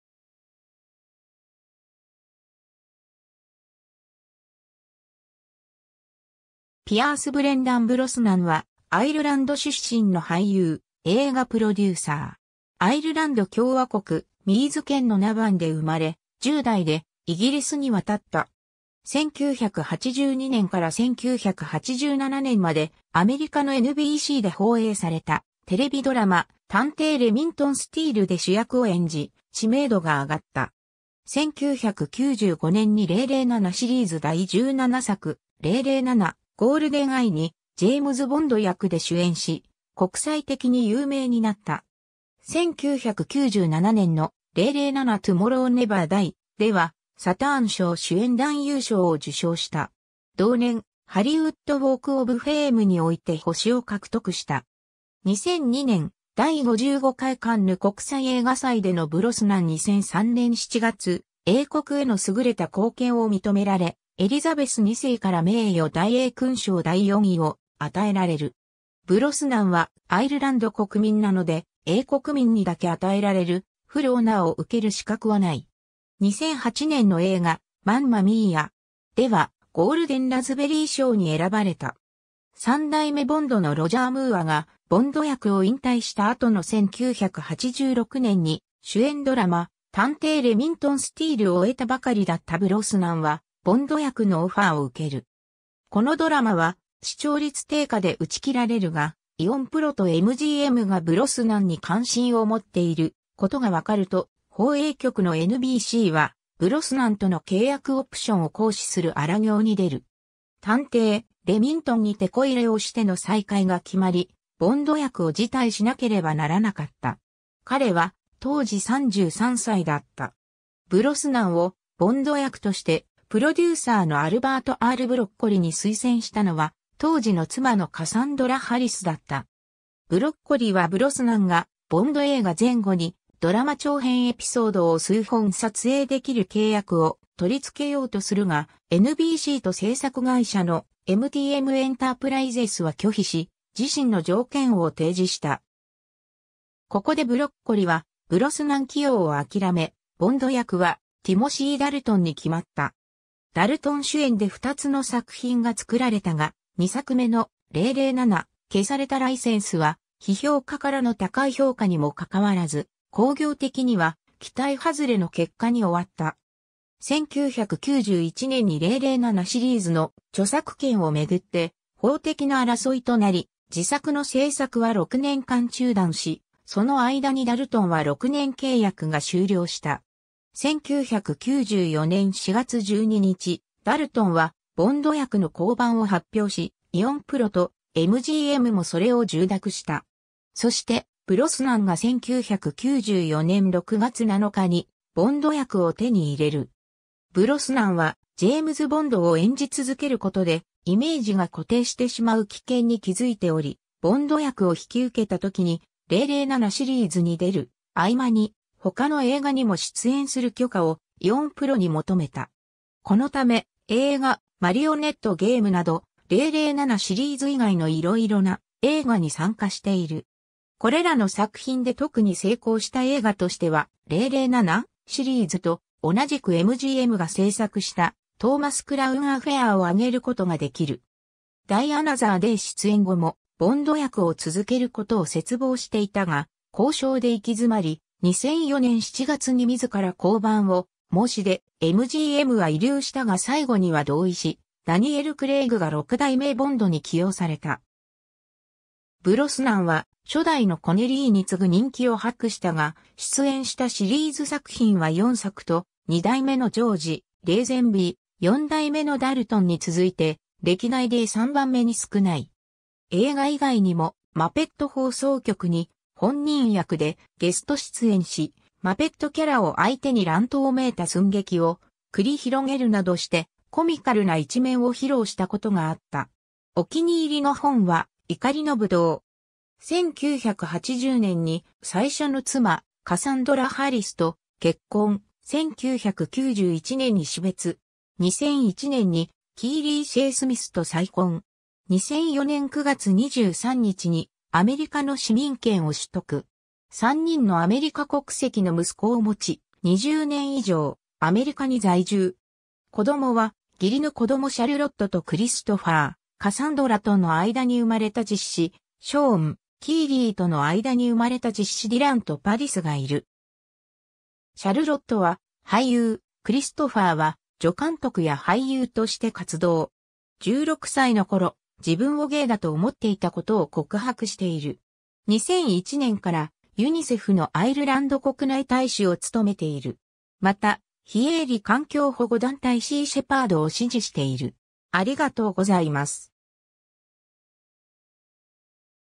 ピアース・ブロスナン
クリエイティヴ・アーティスツ・エージェンシー
ジェームズ・ボンド
ピアース・ブレンダン・ブロスナン（Pierce Brendan Brosnan OBE, 1953年5月16日 - ）は、アイルランド出身の俳優、映画プロデューサー。
 アイルランド共和国ミーズ県のナヴァンで生まれ、10代でイギリスに渡った。
1982年から1987年までアメリカのNBCで放映されたテレビドラマ『探偵レミントン・スティール』で主役を演じ、知名度が上がった。
 1995年に007シリーズ第17作『007 ゴールデンアイ』にジェームズ・ボンド役で主演し、国際的に有名になった。
1997年の『007 トゥモロー・ネバー・ダイ』では、サターン賞主演男優賞を受賞した。
同年、ハリウッド・ウォーク・オブ・フェームにおいて星（星形のプレート）を獲得した。
 2002年、第55回カンヌ国際映画祭でのブロスナン 2003年7月、英国への優れた貢献を認められ、エリザベス2世から名誉大英勲章第4位を与えられる。
ブロスナンはアイルランド国民なので、英国民にだけ与えられる「full honour」を受ける資格はない。
 2008年の映画『マンマ・ミーア!』では、ゴールデンラズベリー賞に選ばれた。
 3代目ボンドのロジャー・ムーアがボンド役を引退したあとの1986年に、主演ドラマ『探偵レミントン・スティール』を終えたばかりだったブロスナンはボンド役のオファーを受ける。
このドラマは視聴率低下で打ち切られるが、イオン・プロとMGMがブロスナンに関心を持っていることが分かると、放映局のNBCはブロスナンとの契約オプションを行使する荒業に出る。
『探偵レミントン』にテコ入れをしての再開が決まり、ボンド役を辞退しなければならなかった。
彼は当時33歳だった。
ブロスナンをボンド役としてプロデューサーのアルバート・R・ブロッコリに推薦したのは、当時の妻のカサンドラ・ハリスだった。
ブロッコリはブロスナンがボンド映画前後に、ドラマ長編エピソードを数本撮影できる契約を取り付けようとするが、NBCと制作会社のMTM Enterprisesは拒否し、自身の条件を提示した。
ここでブロッコリはブロスナン起用を諦め、ボンド役はティモシー・ダルトンに決まった。
ダルトン主演で2つの作品が作られたが、2作目の『007 消されたライセンス』は、批評家からの高い評価にもかかわらず、興行的には期待外れの結果に終わった。
1994年4月12日、ダルトンはボンド役の降板を発表し、イオン・プロとMGMもそれを受諾した。
そしてブロスナンが1994年6月7日にボンド役を手に入れる。
 ブロスナンは、ジェームズ・ボンドを演じ続けることでイメージが固定してしまう危険に気付いており、ボンド役を引き受けたときに『007』シリーズに出る合間に他の映画にも出演する許可をイオン・プロに求めた。
このため、映画『マリオネット・ゲーム』など『007』シリーズ以外のいろいろな映画に参加している（プロデュース作品も含む）。
これらの作品で特に成功した映画としては、『007』シリーズと同じくMGMが製作した『トーマス・クラウン・アフェアー』をあげることができる。
 『ダイ・アナザー・デイ』出演後もボンド役を続けることを切望していたが、交渉で行き詰まり、2004年7月に自ら降板を申し出、MGMは慰留したが最後には同意し、ダニエル・クレイグが6代目ボンドに起用された。
ブロスナンは初代のコネリーに次ぐ人気を博したが、出演したシリーズ作品は4作と、2代目のジョージ・レーゼンビー（『女王陛下の007』の1作のみ）、4代目のダルトン（『007 リビング・デイライツ』『007 消されたライセンス』の2作）に続いて歴代で3番目に少ない。
 映画以外にも『マペット放送局』に本人役でゲスト出演し、マペットキャラを相手に乱闘めいた寸劇を繰り広げるなどしてコミカルな一面を披露したことがあった。
 お気に入りの本は「怒りの葡萄」。
 1980年に最初の妻カサンドラ・ハリス（『007 ユア・アイズ・オンリー』のボンドガール）と結婚、1991年に死別。
2001年にキー...